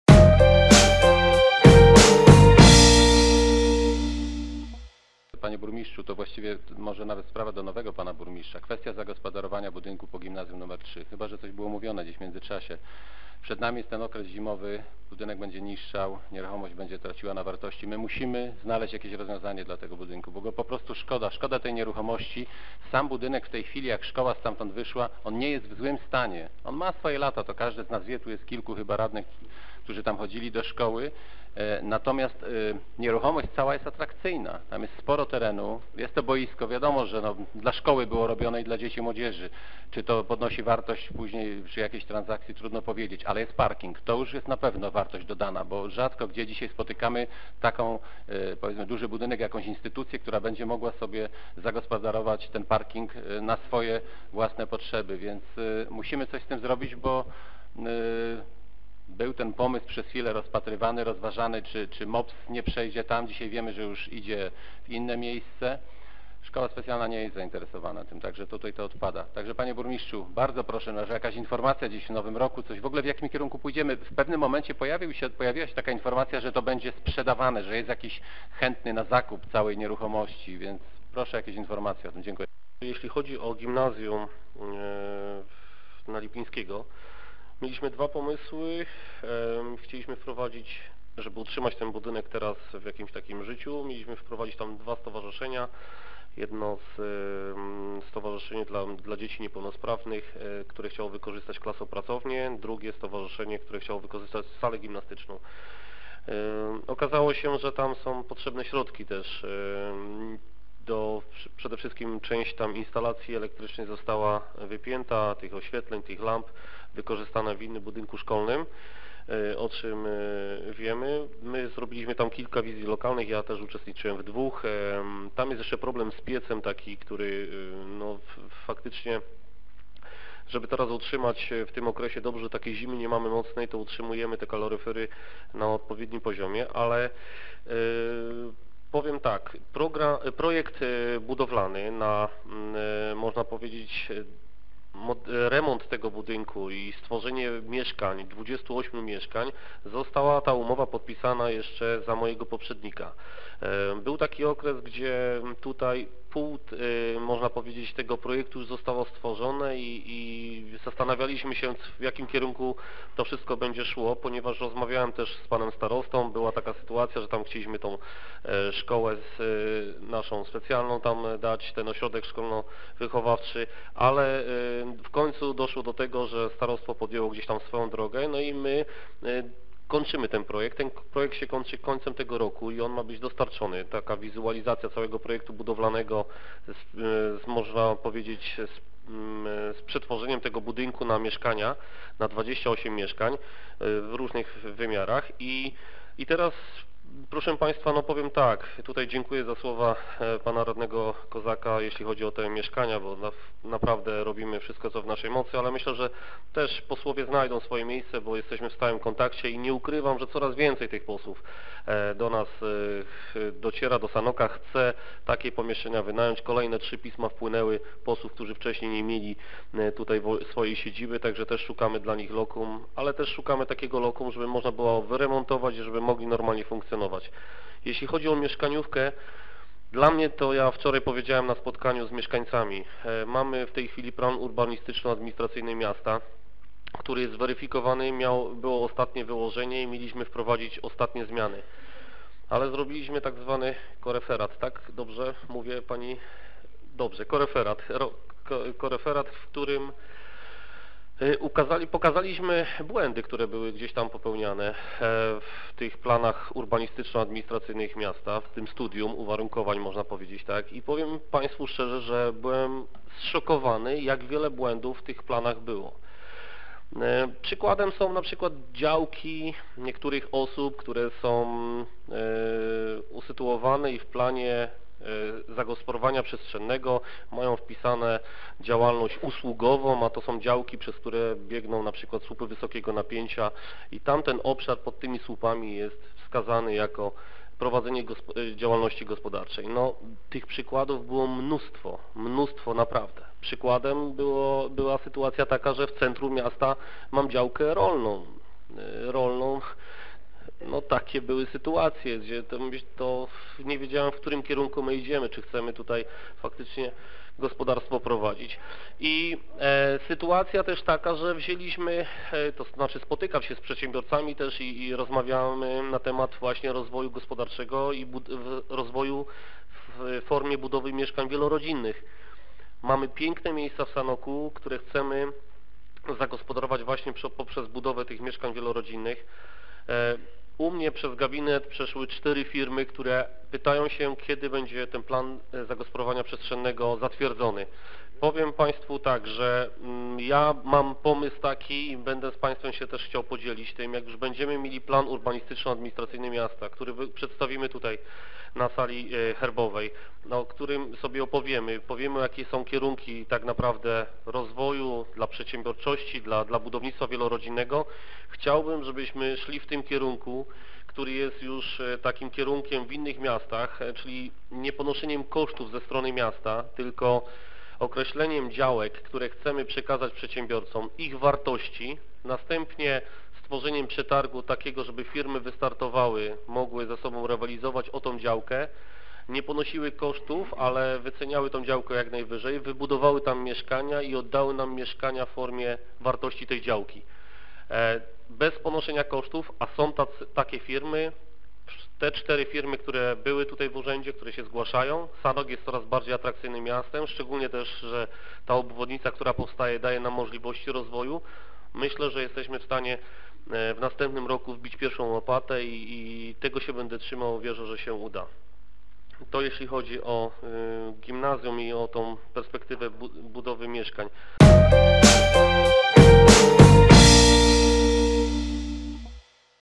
Co z budynkiem szkoły? Problem z piecem i elektryką (VIDEO)
SANOK / PODKARPACIE. Podczas ostatniej sesji Rady Miasta Sanoka radny Maciej Drwięga pytał między innymi o nie zagospodarowany budynek po gimnazjum nr 3 w Sanoku zlokalizowany przy ulicy Lipińskiego w Sanoku. Obiekt stoi pusty i niszczeje. Czy jest pomysł na jego zagospodarowanie? Odpowiedź w materiale filmowym.